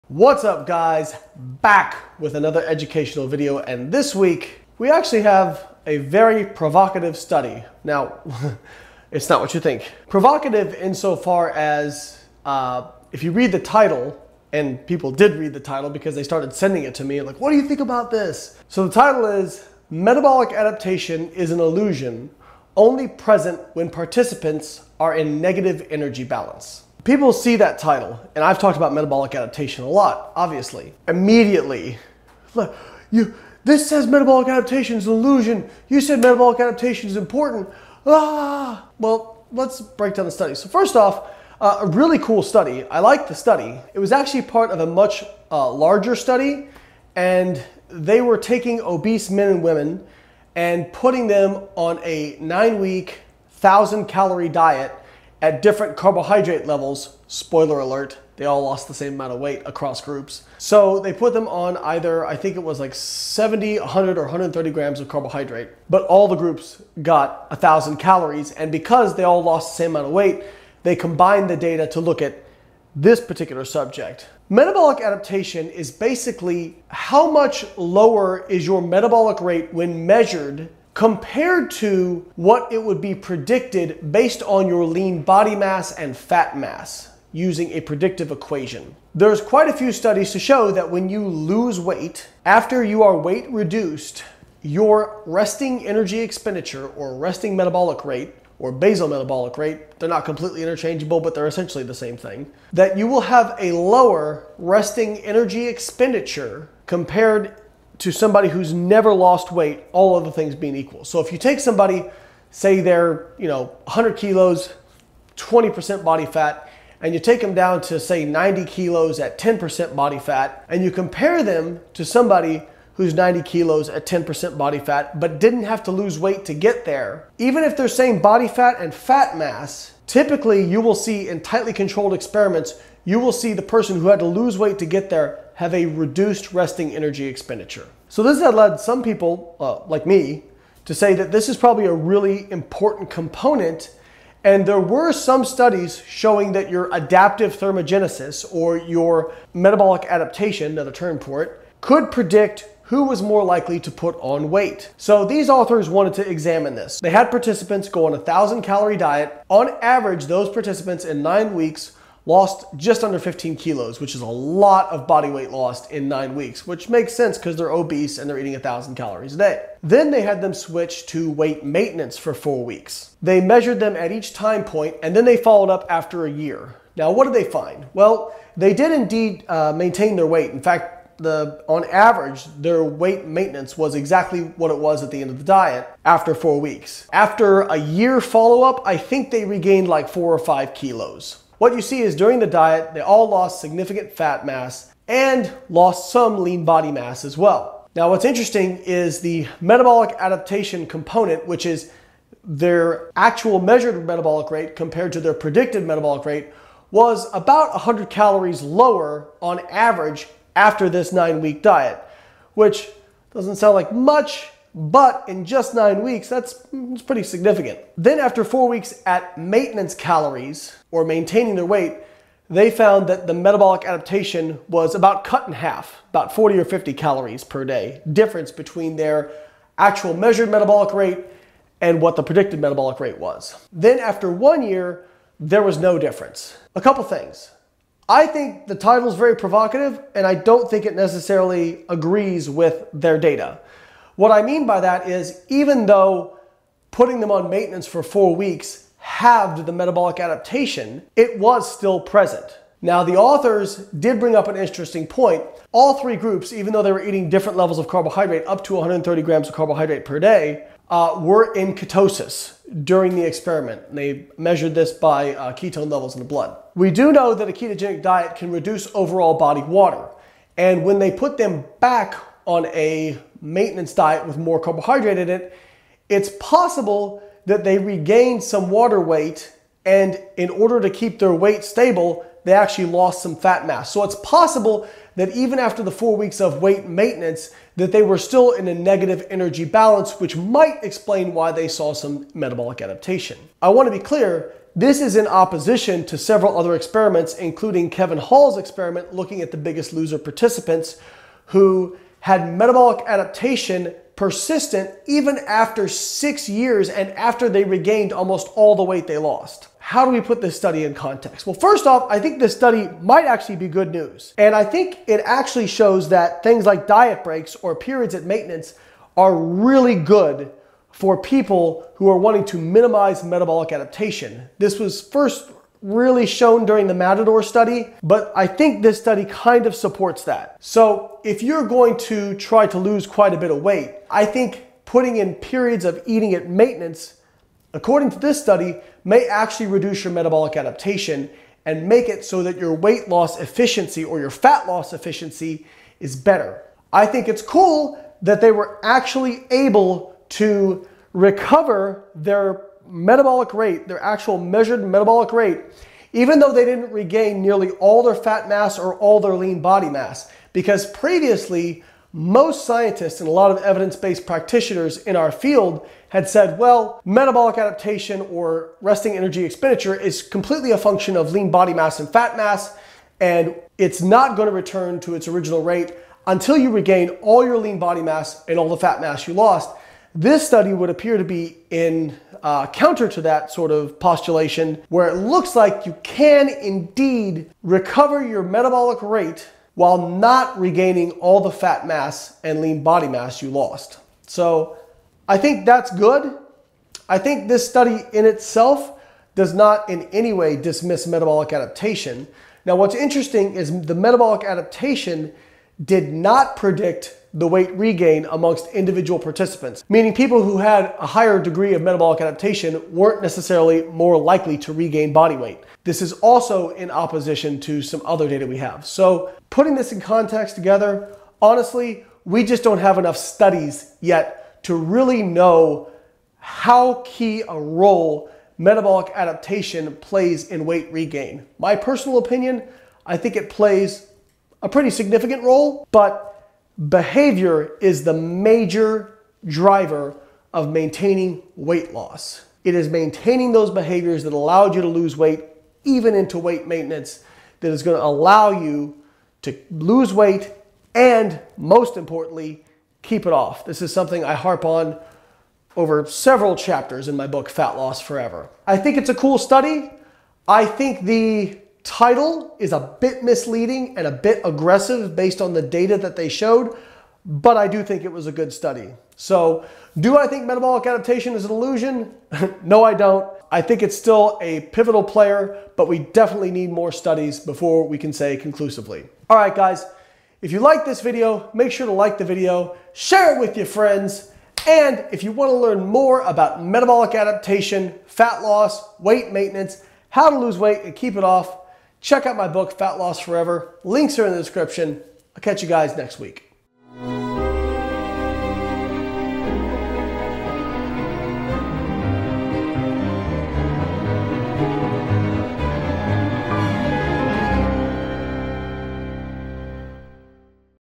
Metabolic Adaptation Is An Illusion
A new study made a lot of headlines with it's title "Metabolic Adaptation is an Illusion." Provocative title and the study itself was extremely well done and interesting, but do the data support the author's conclusions? It's not as clear as you might think.

Get my new nutrition coaching app: Carbon Diet Coach for iOS and android to get custom nutrition coaching for less than $10/month